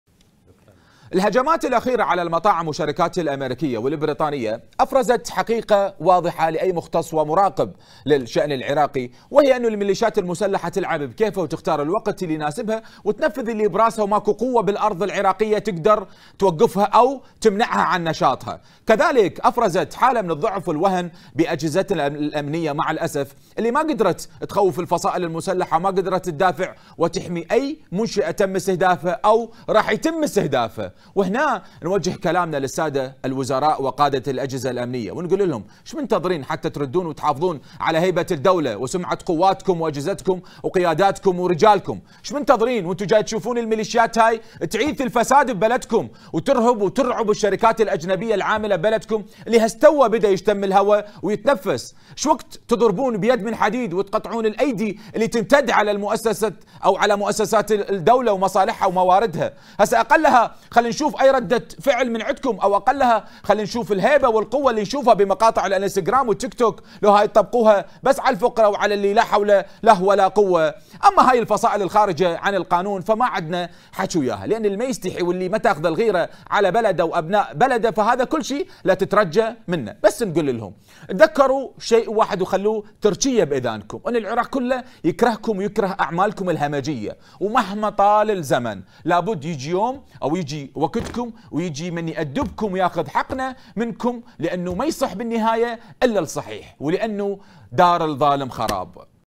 الميليشيات المسلحة تختار الوقت وتنفذ ما تريد وليس هناك قوة على الأرض تستطيع إيقافها.
الميليشيات المسلحة تختار الوقت وتنفذ ما تريد وليس هناك قوة على الأرض تستطيع إيقافها.. لكن تذكروا أن العراق "يكرههم ويكره أعمالهم الهمجية"